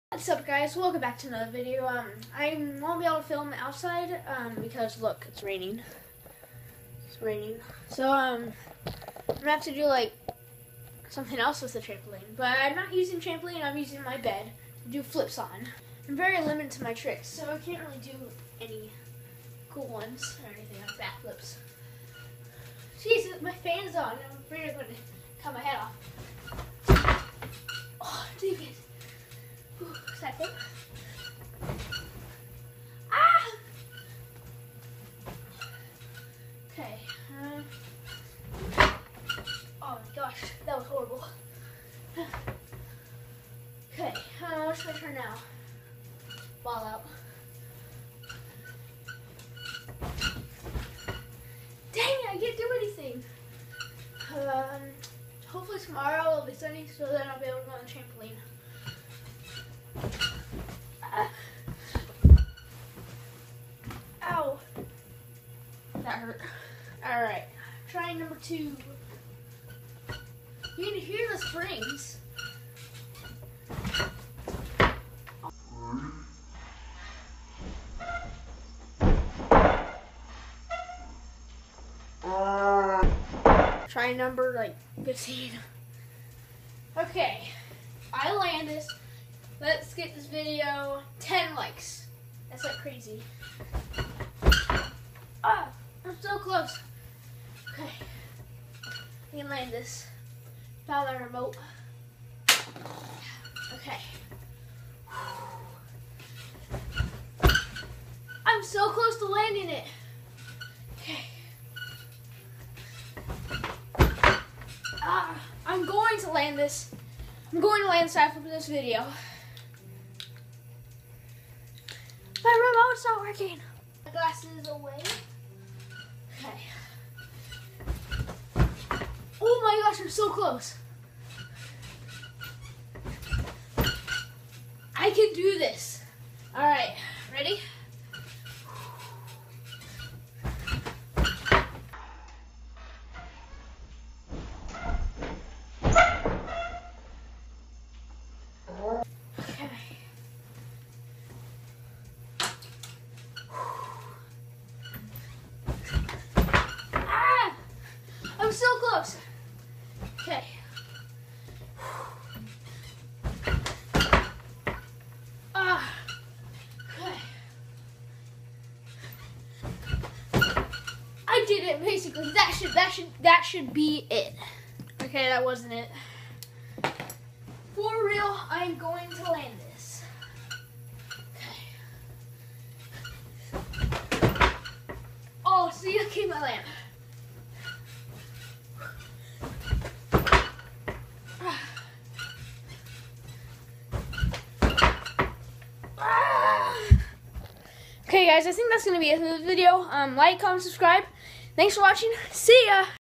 Landing A Sideflip On My Bed! (Bed Trampoline Tricks)
Today was rainy, and I wanted to jump on my trampoline, so, I did tricks in my bed, which was basically like the trampoline... enjoy!